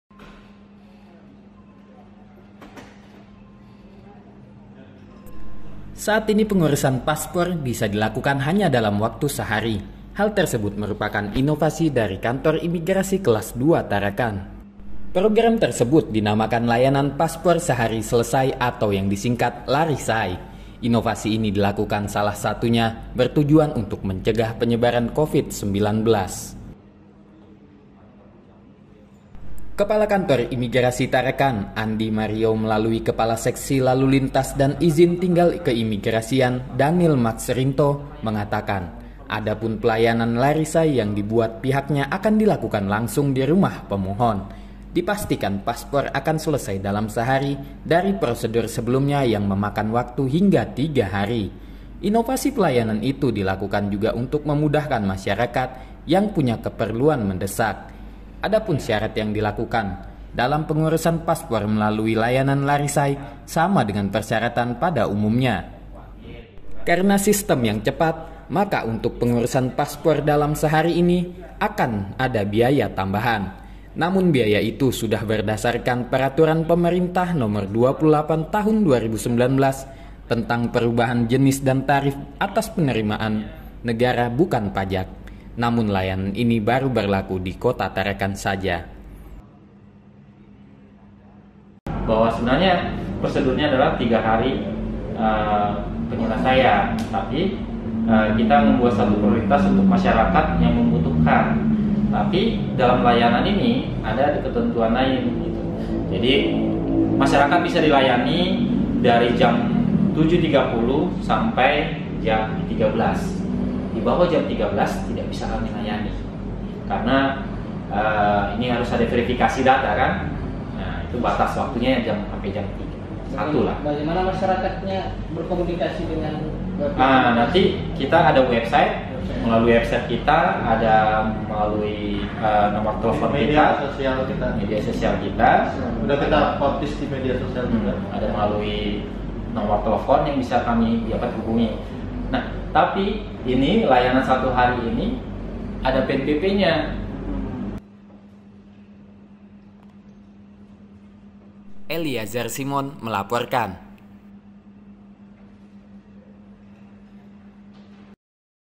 Luncurkan Program ‘Lari Say', Pembuatan Paspor Hanya Sehari
Saat ini kepengurusan paspor bisa dilakukan hanya dalam sehari. Hal tersebut merupakan inovasi dari Kantor Imigrasi Kelas II Tarakan. Program tersebut merupakan Layanan Paspor Sehari Selesai atau yang disingkat ‘Lari Say’. Inovasi ini dilakukan salah satunya bertujuan untuk mencegah penyebaran Covid-19.
Reporter: Eliazar Simon